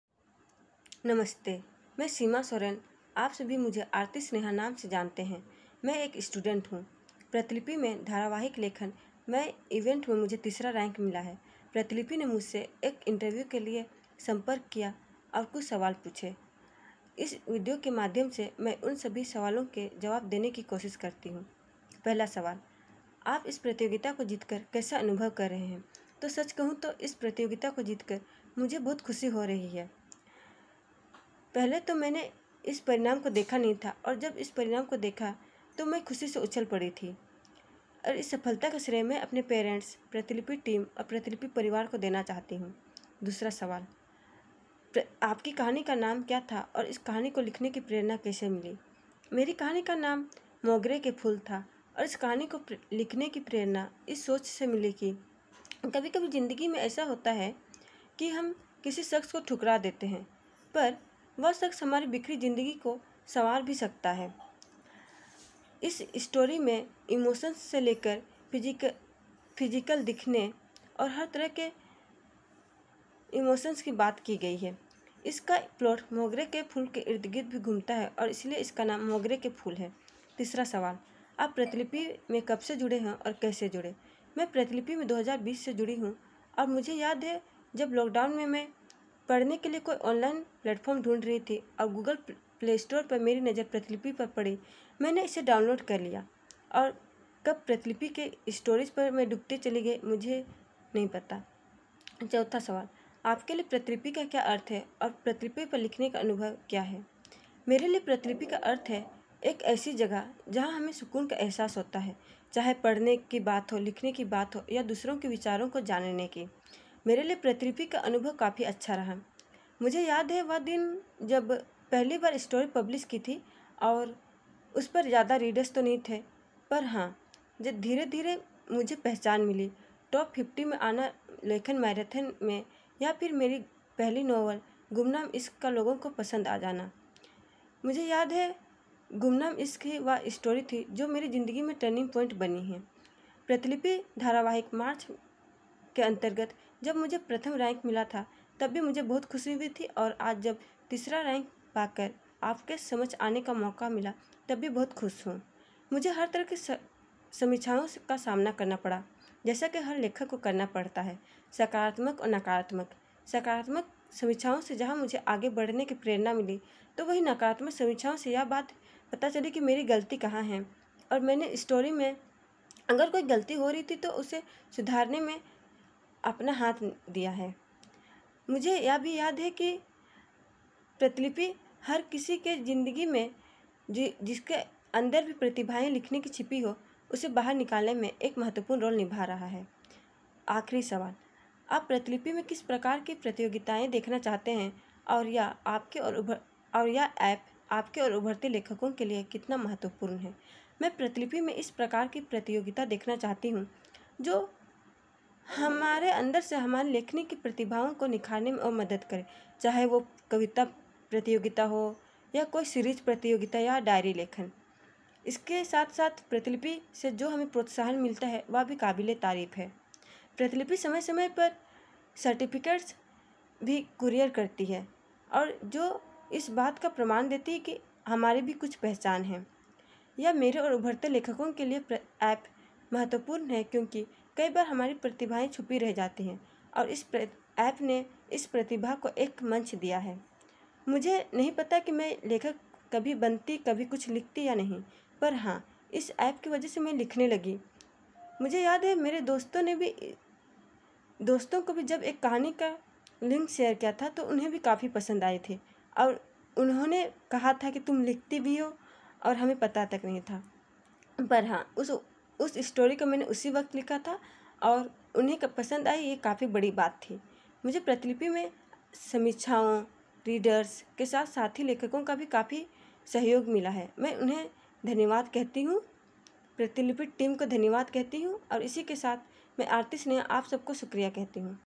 धारावाहिक लेखन प्रतियोगिता__तृतीय विजेता_आरती स्नेहा
धारावाहिक लेखन प्रतियोगिता तृतीय पुरस्कार विजेता लेखिका आरती स्नेहा जी का साक्षात्कार